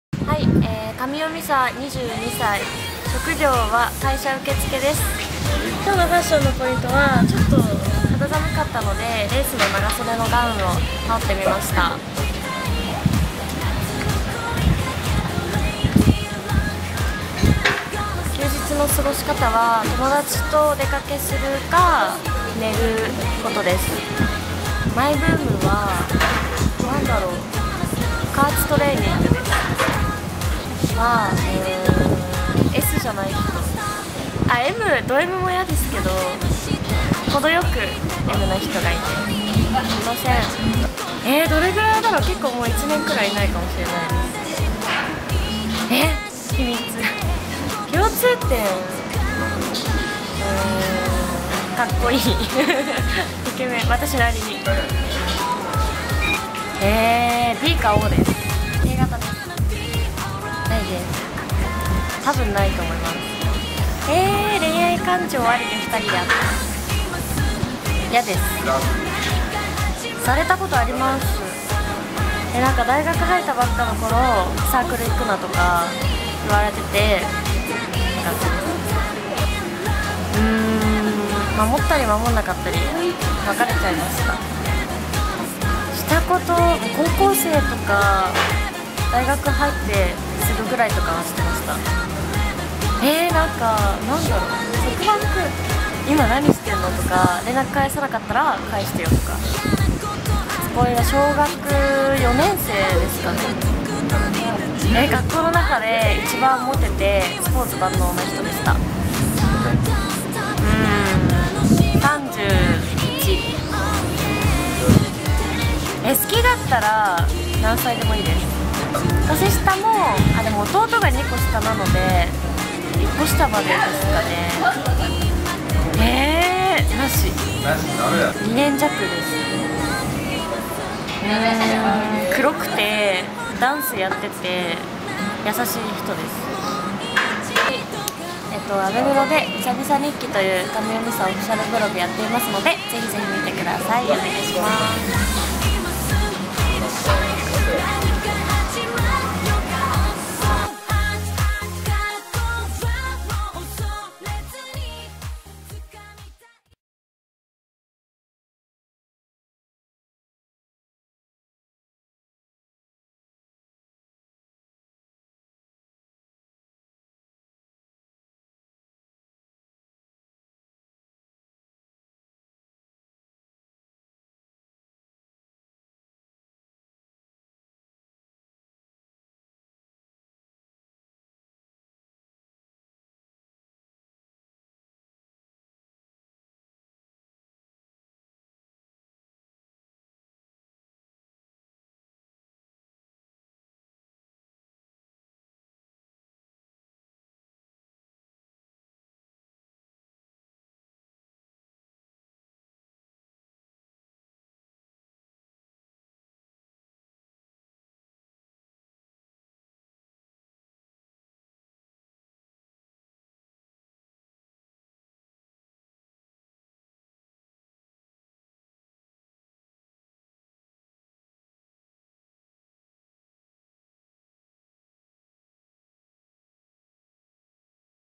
神尾 美沙 インタビュー／好きな男性のタイプは？
渋谷ギャルに恋愛・ファッションインタビュー。